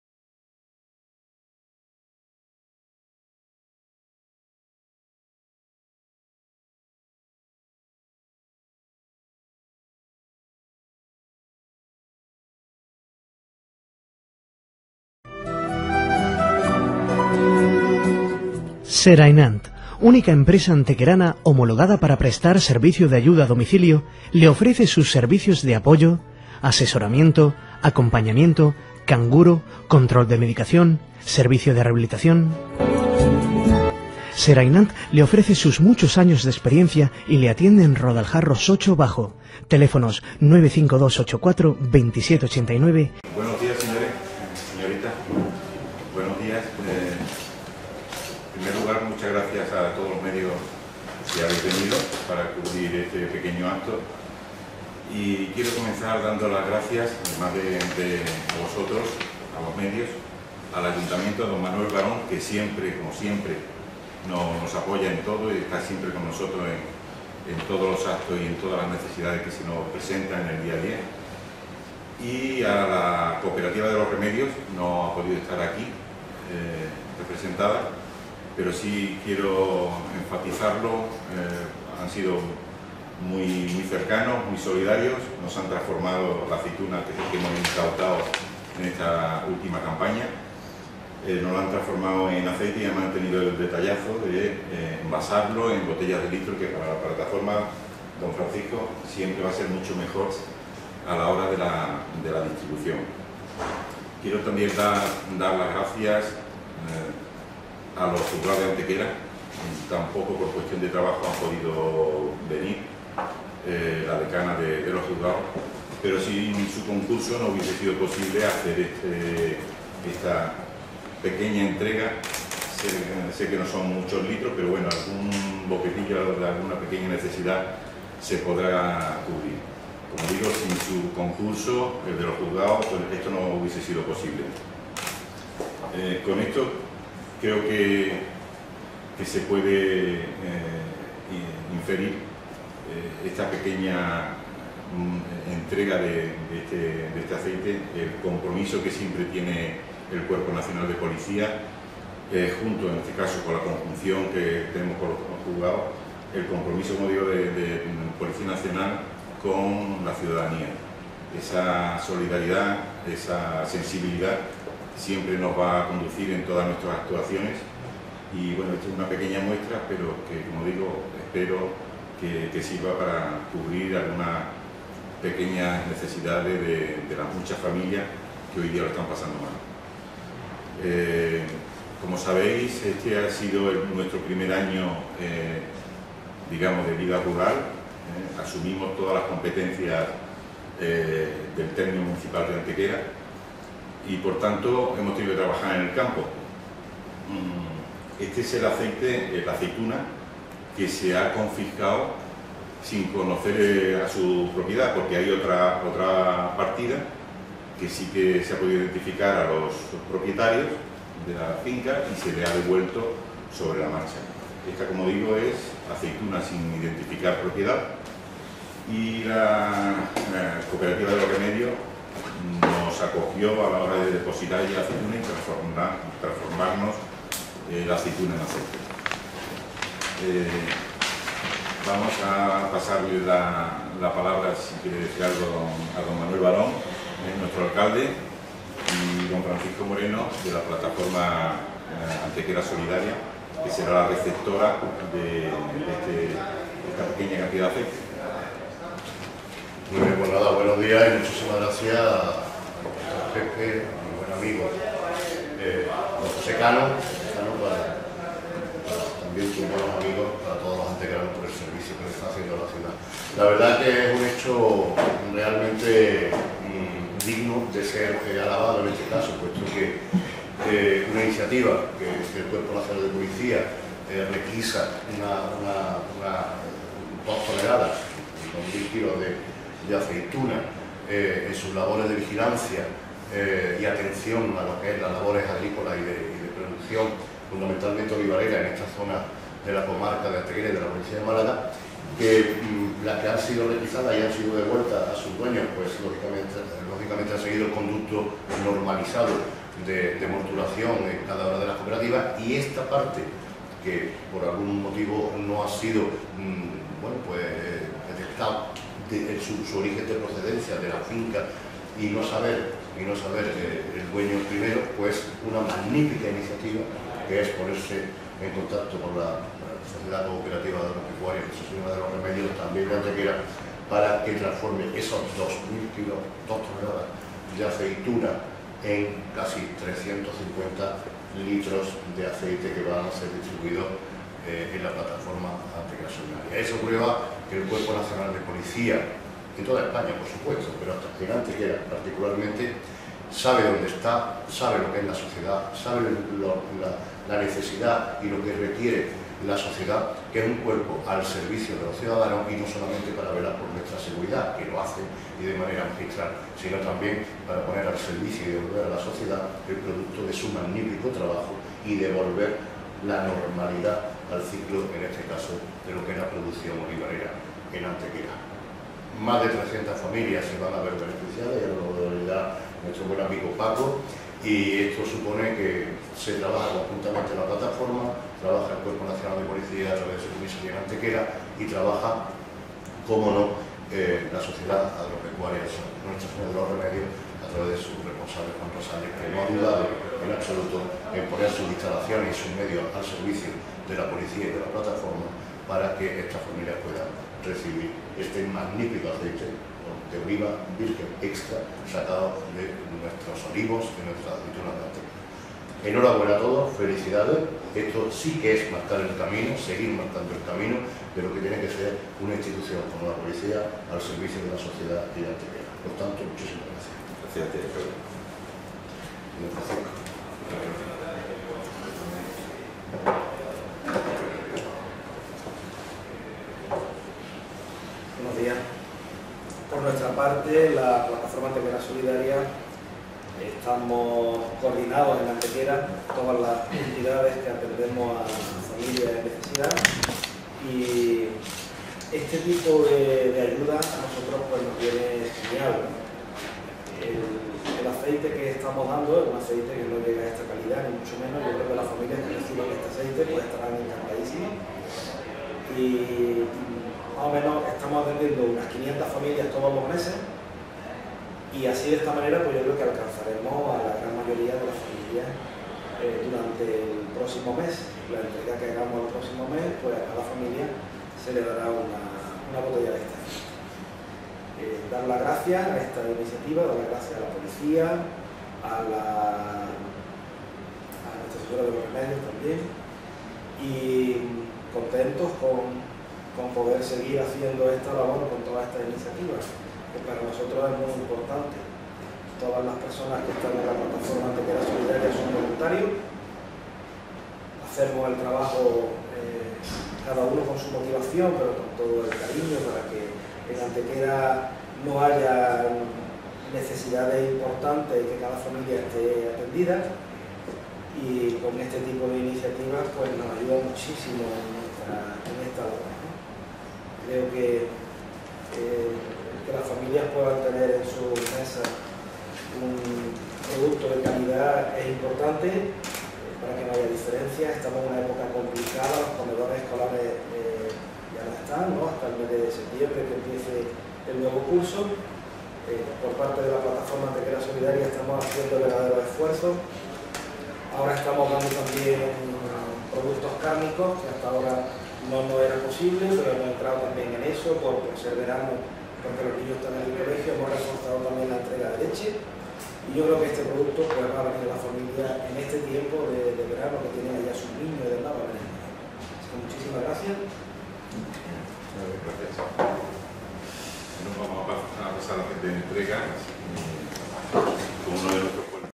Aceitunas Solidarias Antequera
La Policía Nacional de Antequera entrega 345 litros de aceite de oliva virgen extra, de aceitunas incautadas en robos agrícolas, al banco de alimentos local. Rueda de prensa el jueves 21 de julio de 2016 en la Comisaría de Antequera.